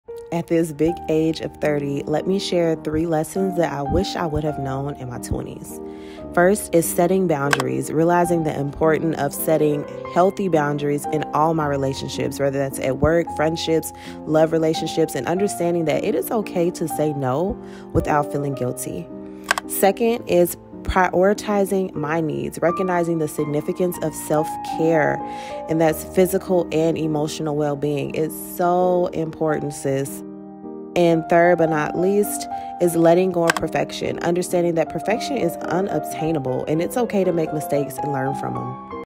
Three lessons I learned in my 20s
Growing & Glowing 101 ✨

1. Setting boundaries: let’s admit it learning to say no or putting yourself first is most times easier said than done. But you have to learn to do so without feeling guilty.

2. Prioritizing your needs: as a mother finding time for myself can sometimes feel like I’m neglecting people or things I shouldn’t. But you can’t pour from an empty cup, so fill yourself first so your can fill others

3. Letting go of perfection: this is seriously one thing I’m so glad I’ve grown out off. Trying to appear perfect and stressing yourself about being perfect or making perfect decisions will drive you crazy. Each L in life is lesson, rather you see it or not.